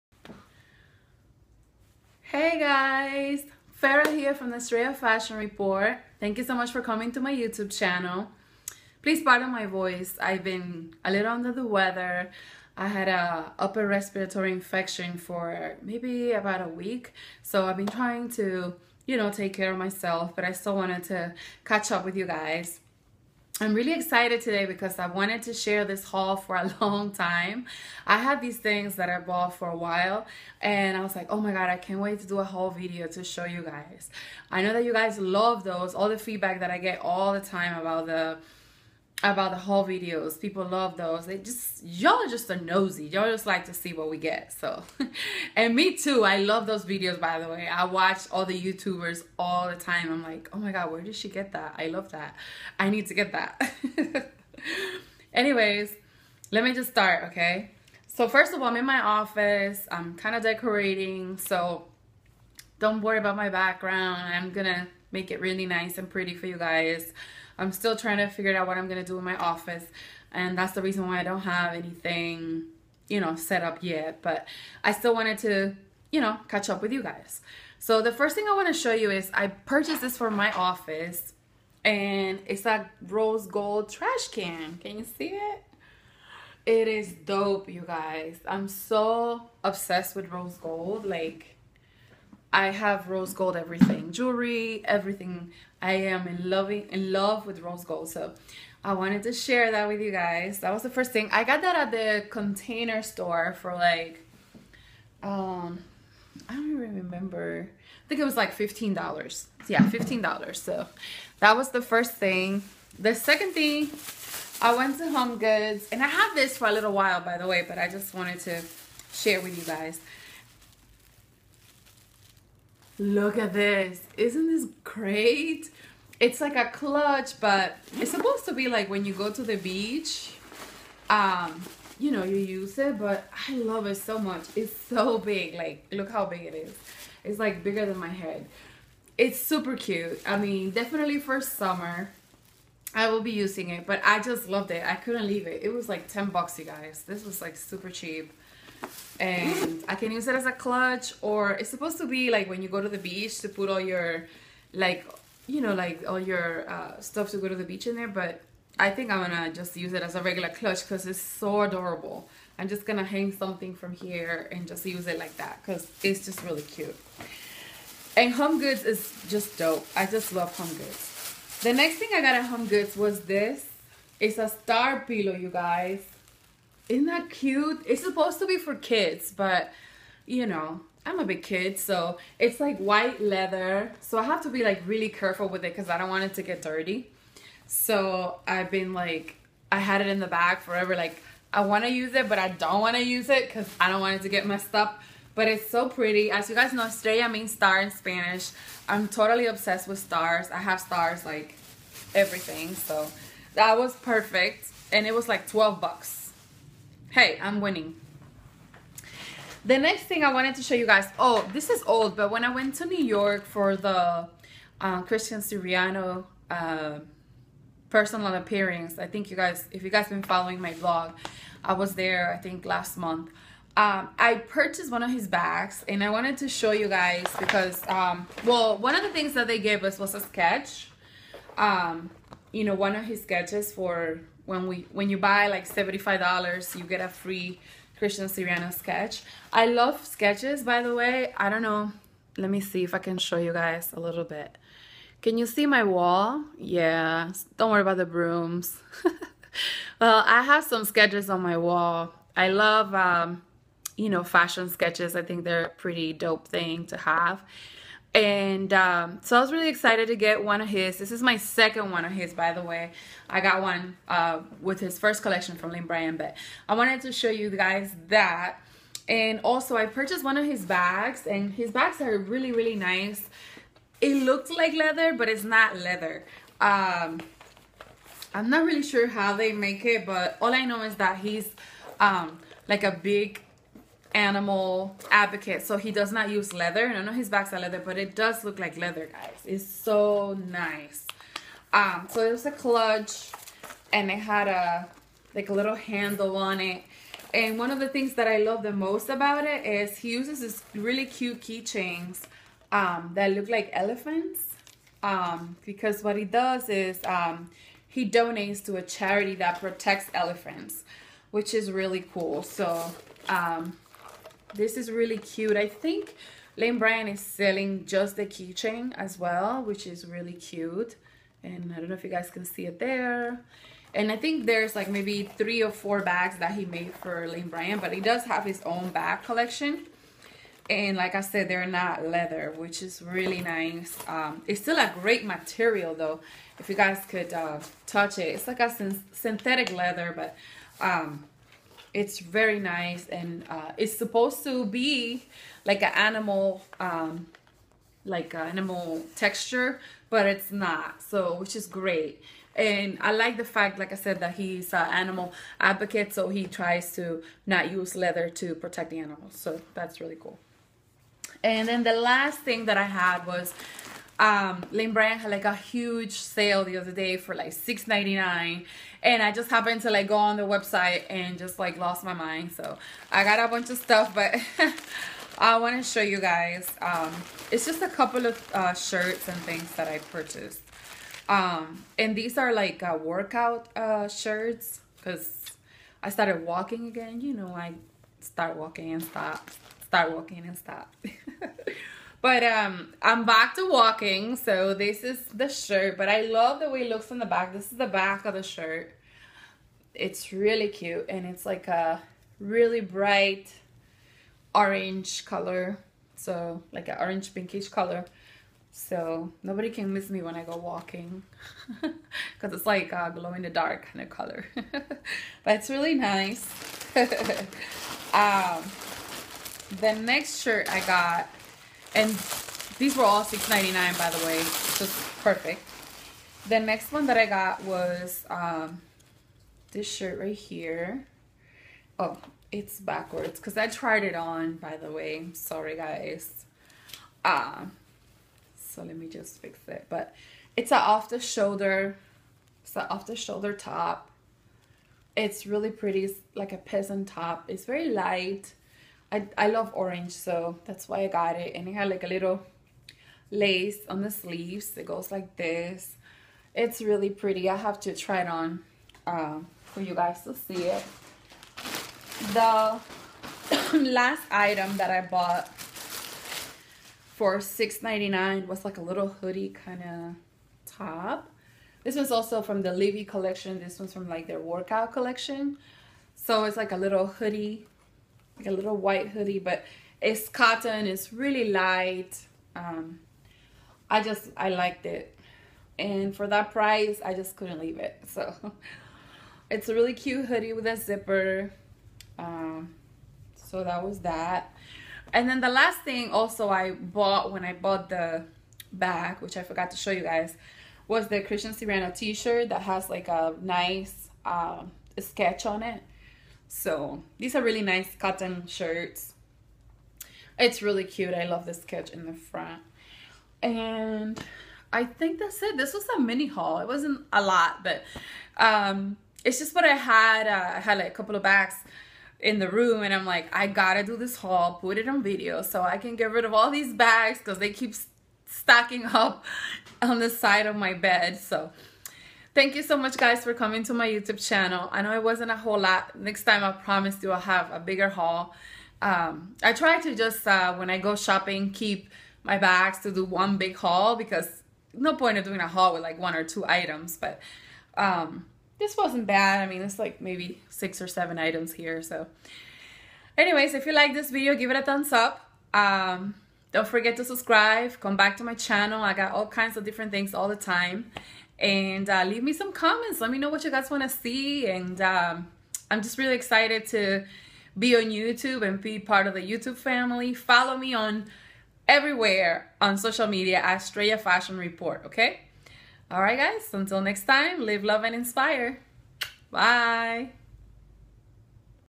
October 2016 Haul | Lane Bryant, Homegoods & The Container Store
Hola Estrellitas, Check out my Haul Video featuring items from Lane Bryant, Homegoods and The Container Store.
Farrah
XOXO